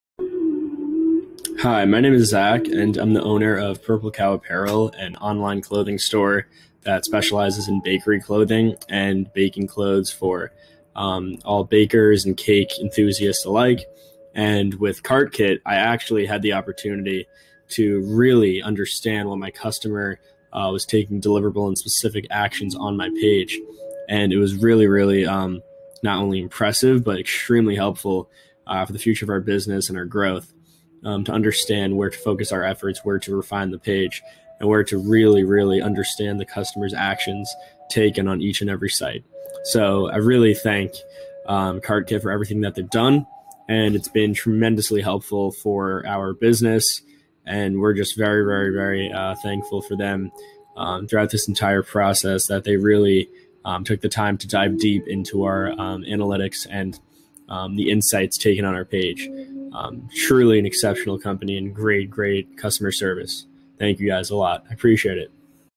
Zach Rubin - Better Replay Testimonial
Zach: Hi, my name is Zach, and I'm the owner of Purple Cow Apparel, an online clothing store that specializes in bakery clothing and baking clothes for all bakers and cake enthusiasts alike.

And with CartKit, I actually had the opportunity to really understand what my customer was taking deliverable and specific actions on my page, and it was really, really not only impressive, but extremely helpful for the future of our business and our growth, to understand where to focus our efforts, where to refine the page, and where to really, really understand the customer's actions taken on each and every site.

So, I really thank CartKit for everything that they've done, and it's been tremendously helpful for our business, and we're just very, very, very thankful for them throughout this entire process, that they really took the time to dive deep into our analytics and the insights taken on our page.

Truly an exceptional company and great, great customer service. Thank you guys a lot, I appreciate it.